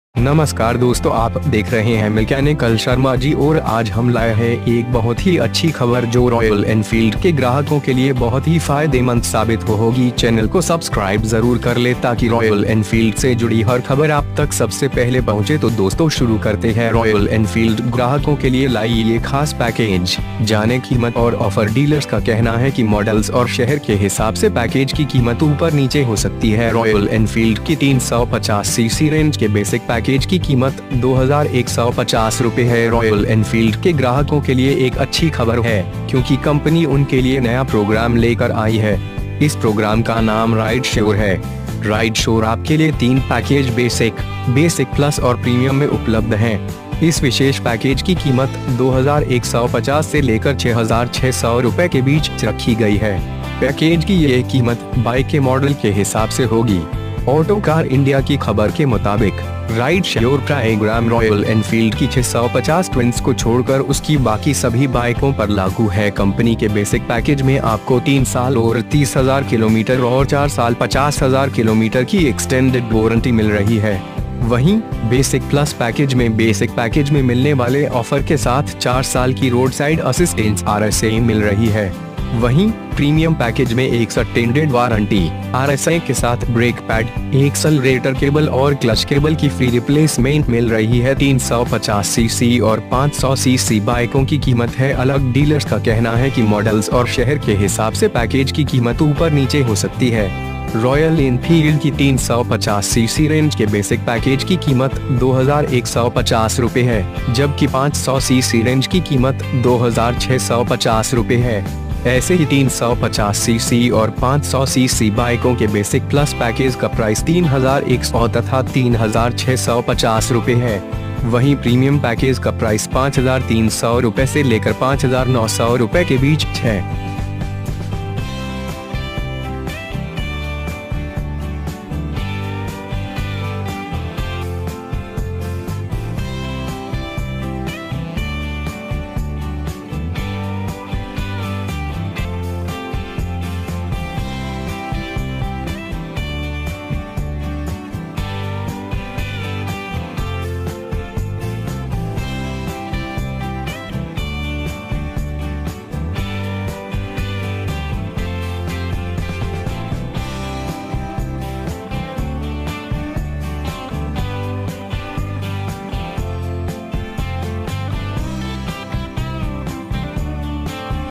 Ride Sure Plan 2020 |Royal Enfield| RSA
नमस्कार दोस्तो इस वीडियो में आपको रॉयल एनफील्ड  के एक प्लान के बारे में बताया गया है जो कि इंटरनेट के द्वारा मुझे जानकारी प्राप्त हुई है
इसमे बताई गई खबर प्राप्त जानकारी के आधार पर दी गयी है
जय हिंद 🇮🇳